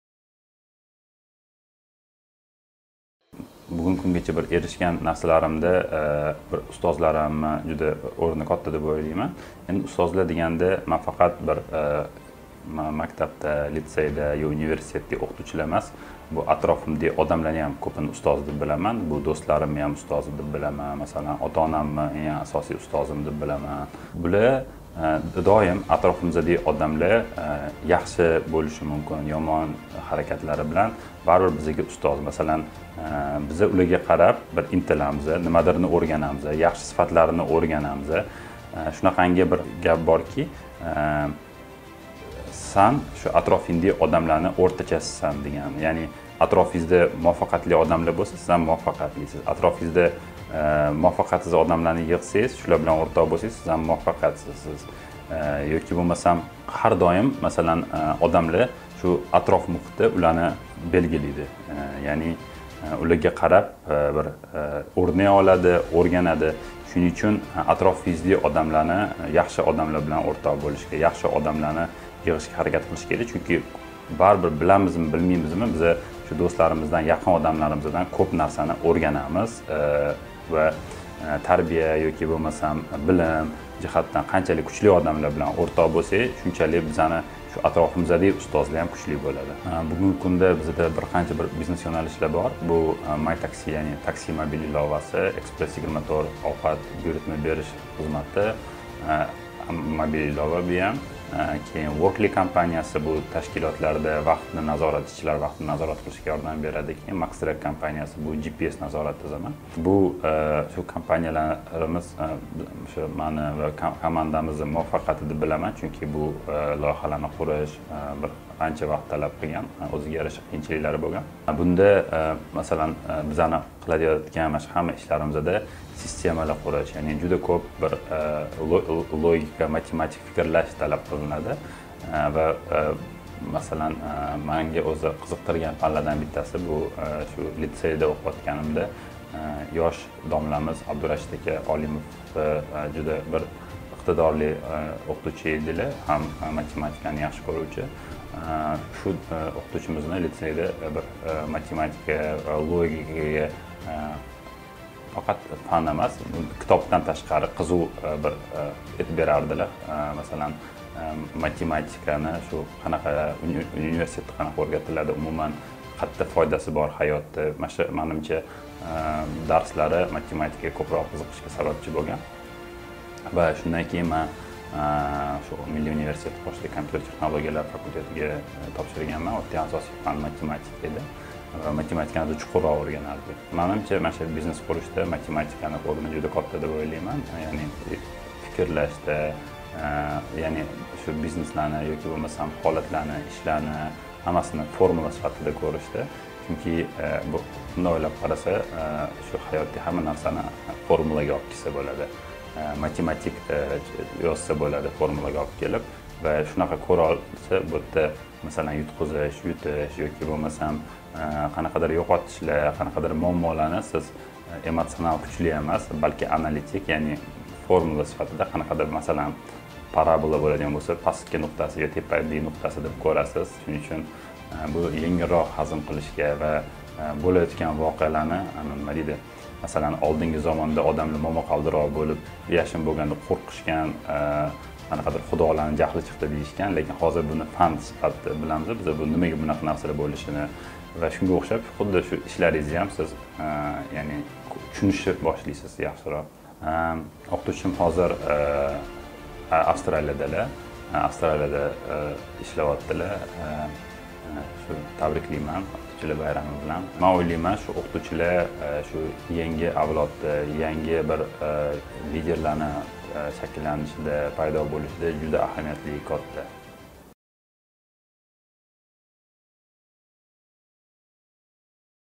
Akmal Payziyevning ustozi kim? | “Ustozimga maktub” loyihasi
Xalq ta`limi vazirligi huzuridagi Respublika taʼlim markazi 1- oktabr – “Ustoz va murabbiylar” kuni bayrami munosabat bilan oʻtkazilgan “Ustozimga maktub” loyihasi doirasida hayotda oʻz oʻrniga ega insonlarning muvaffaqiyatlariga sababchi boʻlgan ustozlar haqida tayyorlangan turkum videoroliklarni namoyish etishda davom etadi.

Express24, Mytaxi, Workly, Maxtrack kabi qator IT-kompaniyalar asoschi Akmal Payziyev bilan tayyorlangan ushbu videorolikni eʼtiboringizga havola etamiz.